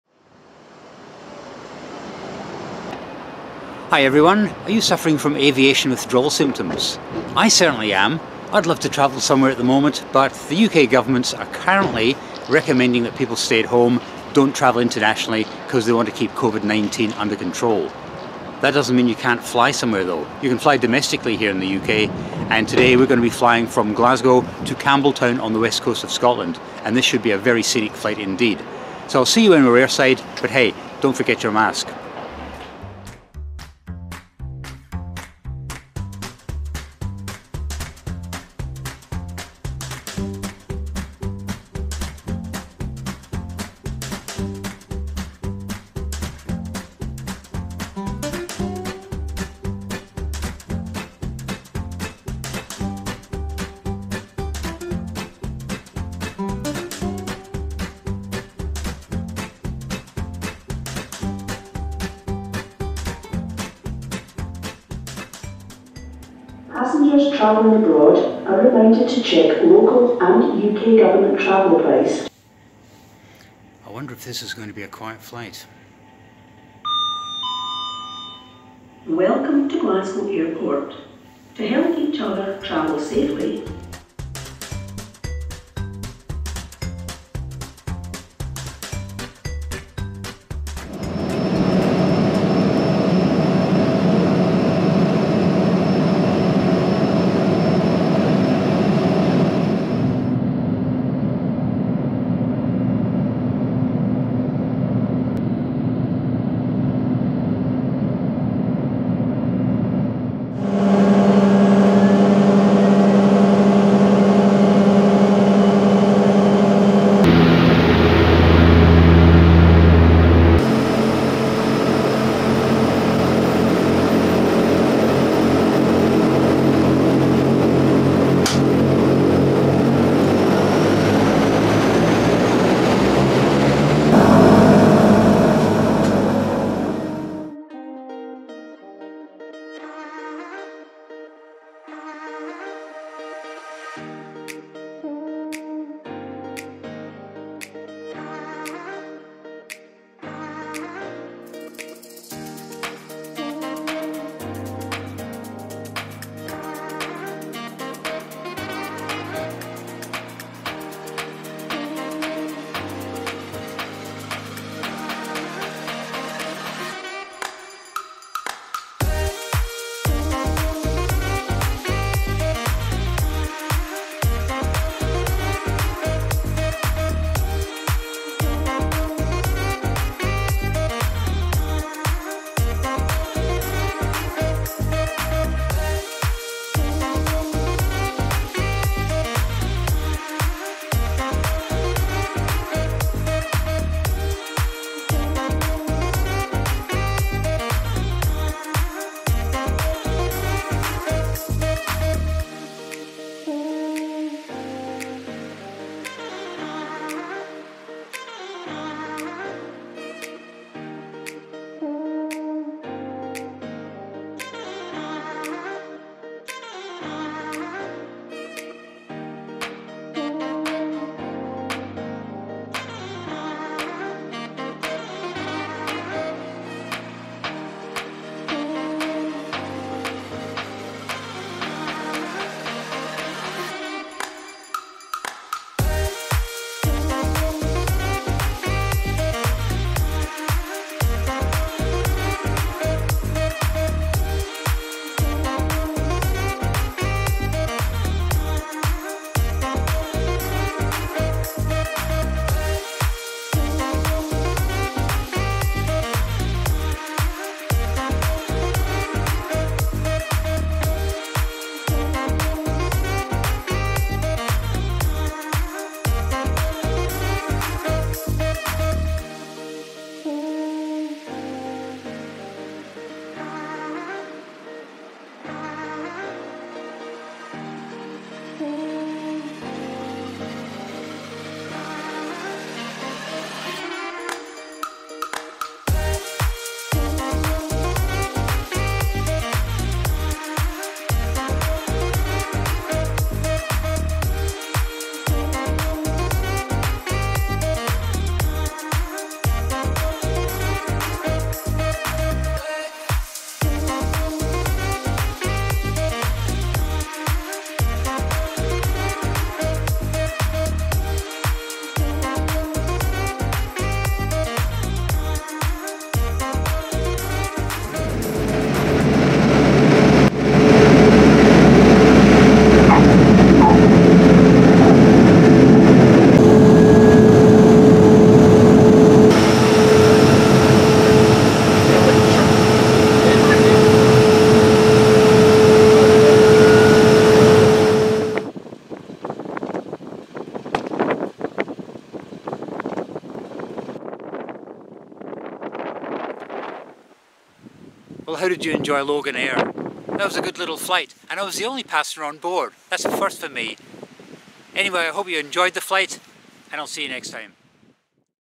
I WAS THE ONLY PASSENGER! - LOGANAIR during Covid-19 - Scenic flight from Glasgow to Campbeltown
I was suffering from aviation withdrawal symptoms. The regional UK governments recommend no foreign travel until the current wave of Covid-19 settles down, so I decided to fly with Loganair from Glasgow to Campbeltown. On this scenic flight... I was the only passenger!